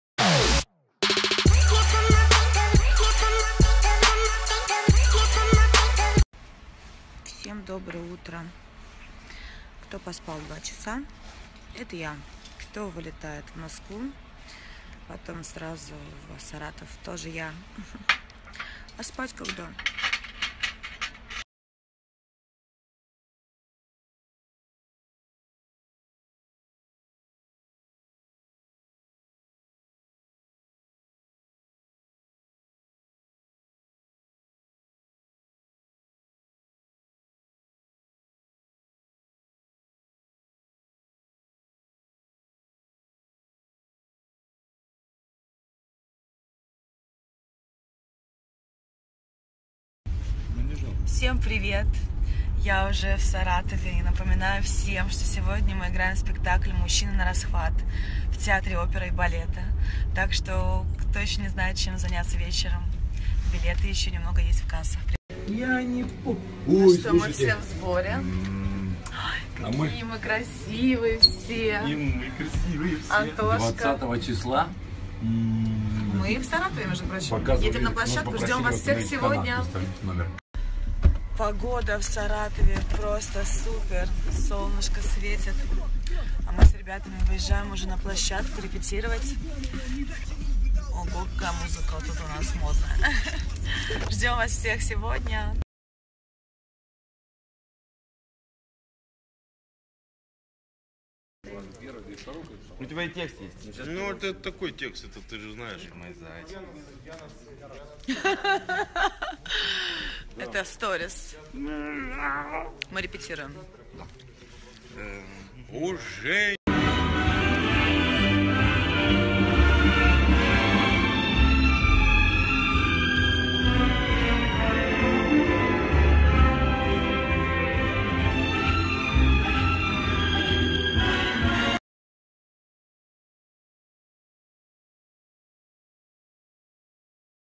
Бузова в Саратове спектакль "Мужчина нарасхват"👏🏻💋
Саратов💋 Мы все на месте. Готовы дарить вам улыбки, смех и отличное настроение 👏🏻Сегодня в 19.00 в театре Оперы и Балета играем для Вас"Мужчина нарасхват" 🙌🏻 До встречи, любимыйзритель 💋